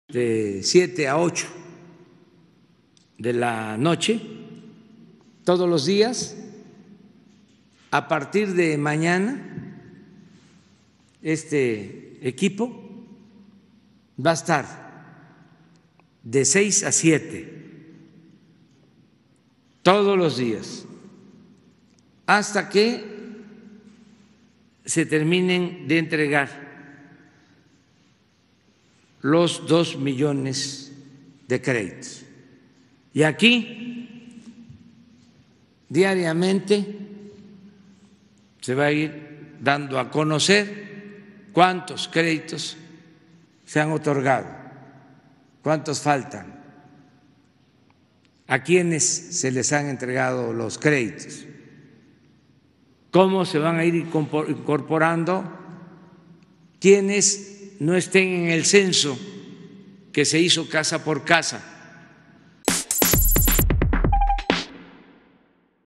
Se informará todos los días sobre la entrega de los 2 millones de créditos: AMLO
«A partir de mañana este equipo estará de 6 a 7 pm todos los días informando, hasta que se terminen de entregar los 2 millones de créditos, se dará a conocer cuántos y a quienes se les han entregado», dijo el presidente López Obrador.